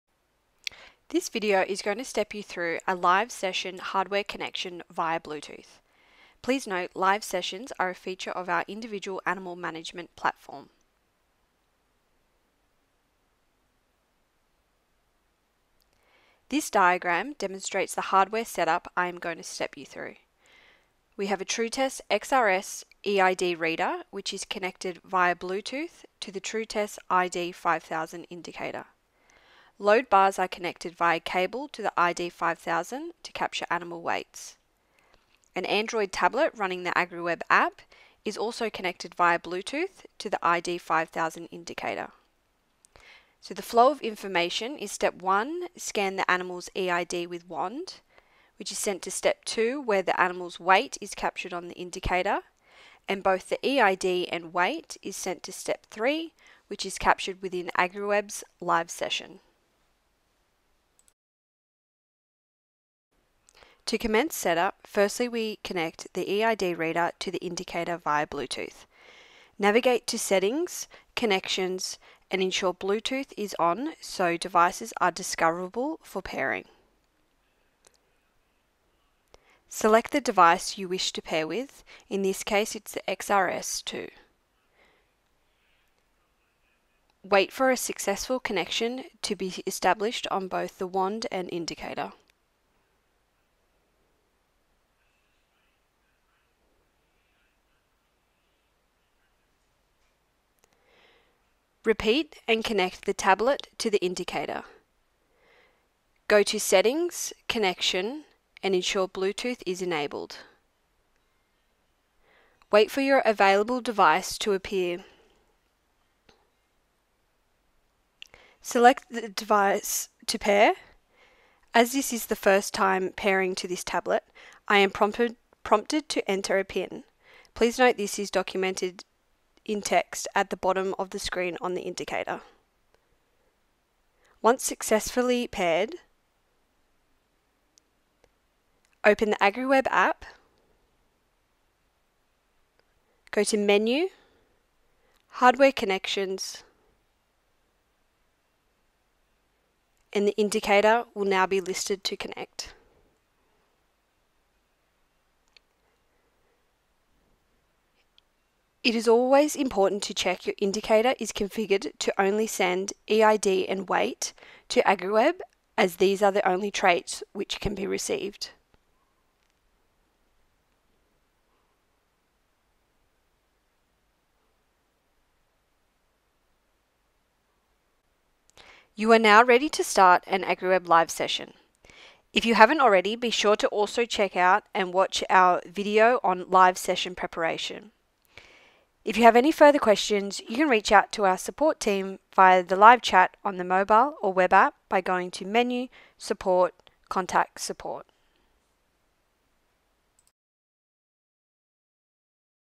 Live Session Bluetooth Hardware Connection
This video will step you through how to connect hardware via bluetooth to AgriWebb when preparing for a live session in the yards.